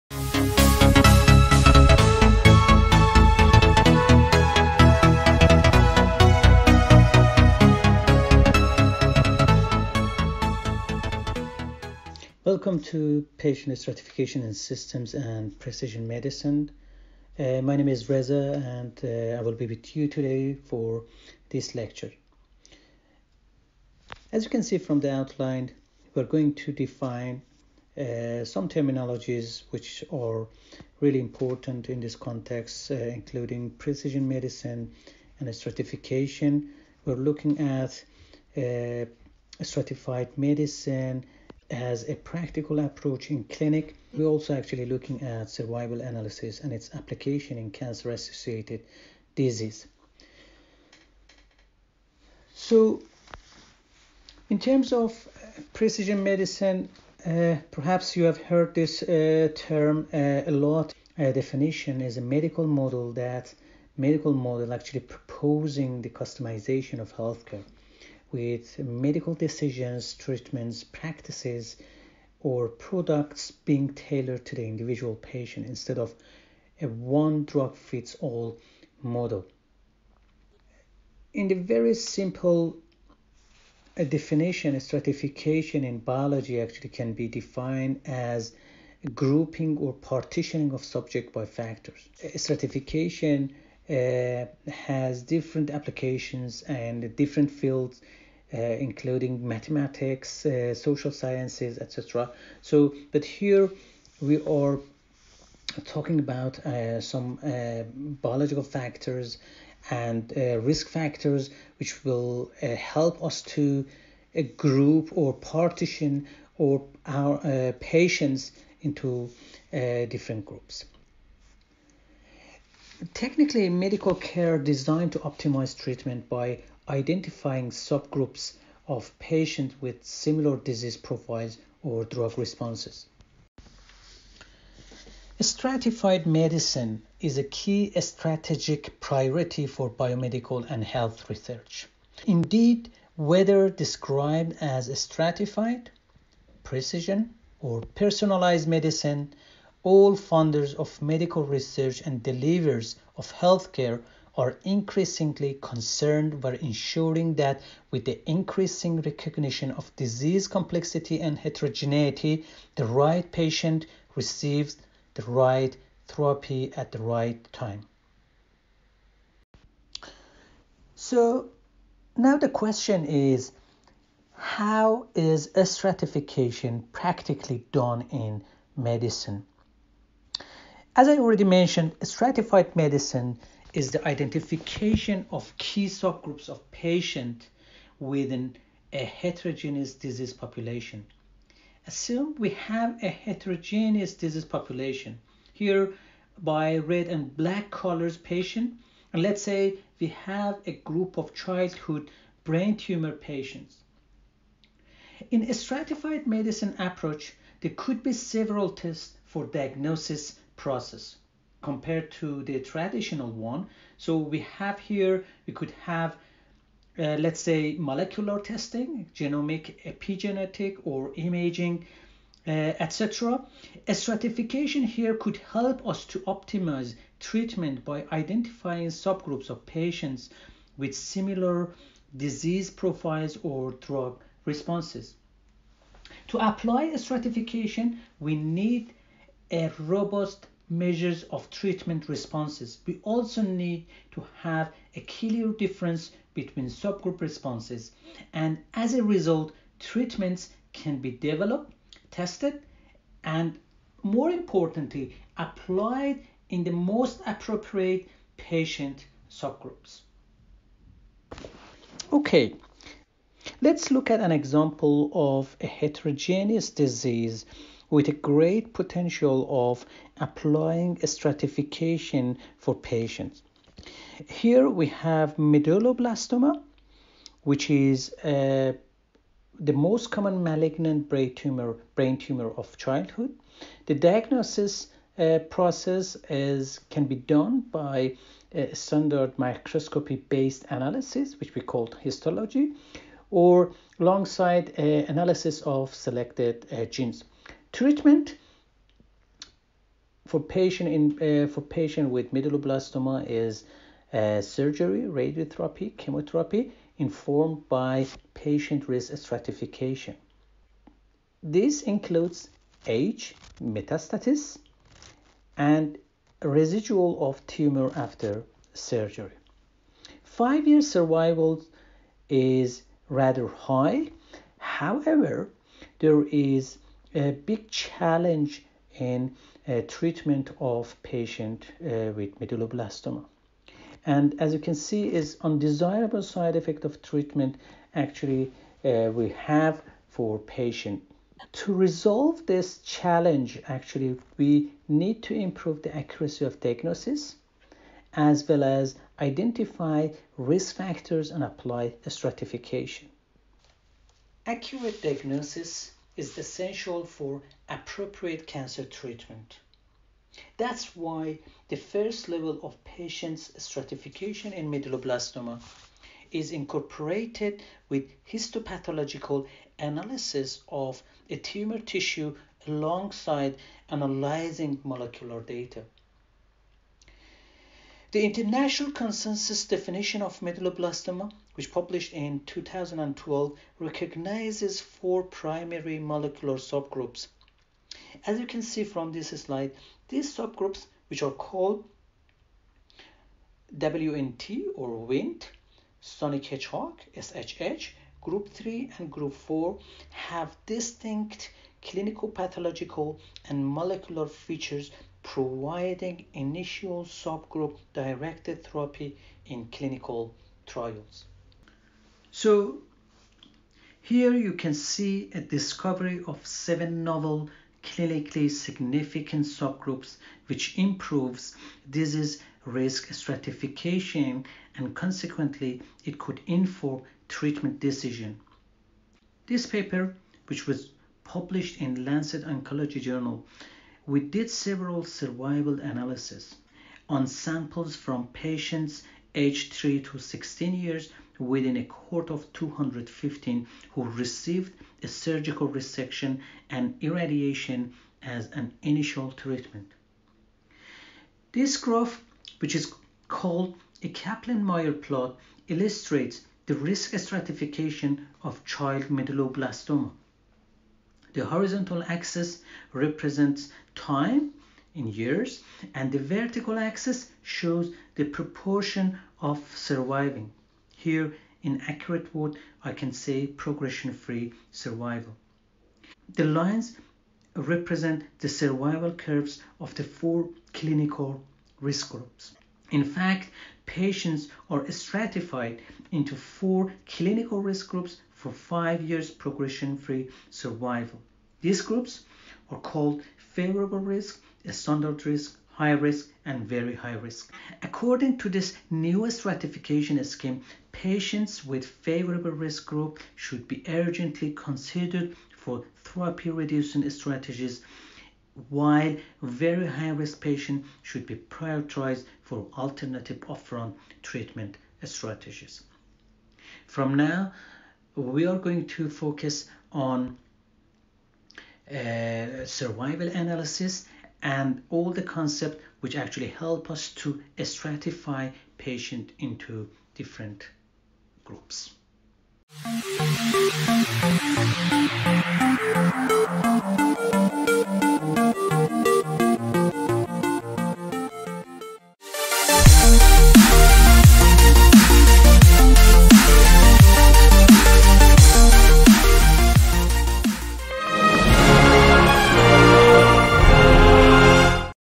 Survival Analysis | Patient Stratification in Systems and Precision Medicine
Patient stratification in systems and precision medicine

Hope you enjoy this educational video.

Survival Analysis | Cox Proportional-Hazards Model | Medulloblastoma | Stratification | Kaplan-Meier plots

Kaplan-Meier

Created by Dr Reza Rafiee; @5MinuteAnalytics @DrRezaRafiee